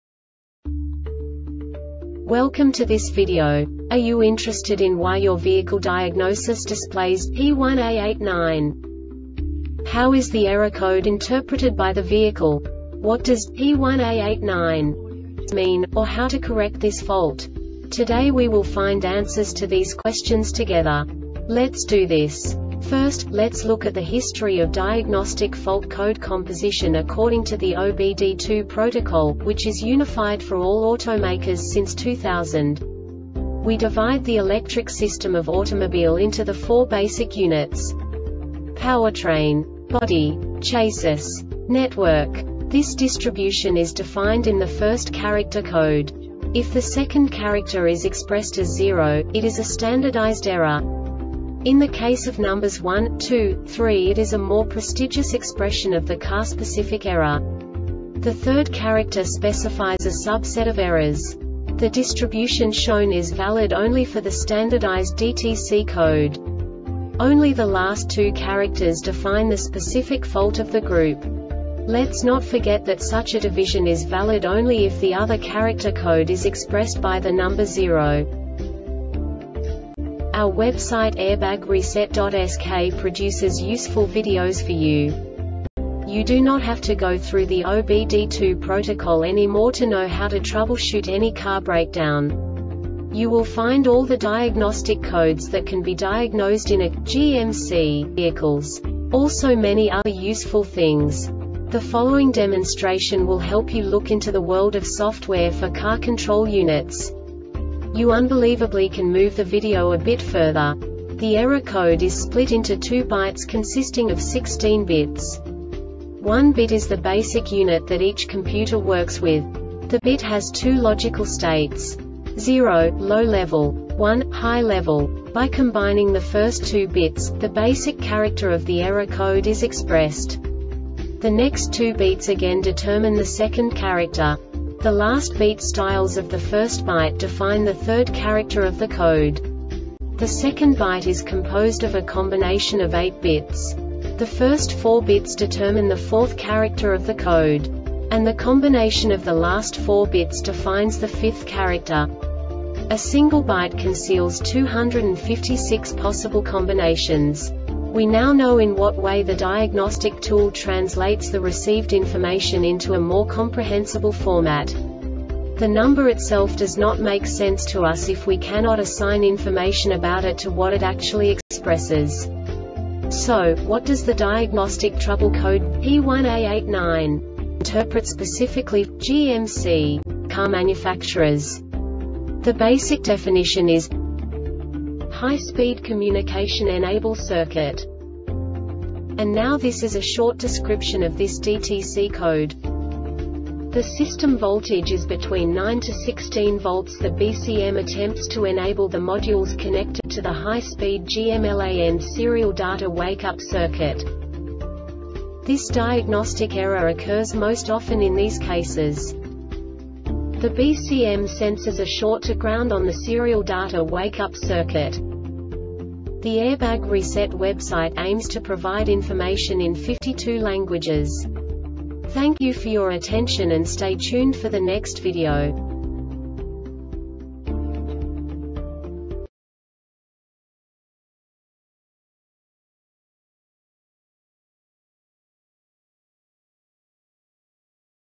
DTC GMC P1A89 Short Explanation
Contents:
0:21 Basic DTC analysis according to OBD2 protocol standard.
1:48 Insight into programming
2:58 A diagnostically understandable description of the Diagnostic Trouble Code
3:05 Basic error definition

Make:
GMC
Code:
P1A89
Definition:
High Speed Communication Enable Circuit
Description:
The system voltage is between 9-16 volts.The BCM attempts to enable the modules connected to the high speed GMLAN serial data wake up circuit.
Cause:
The BCM senses a short to ground on the serial data wake up circuit.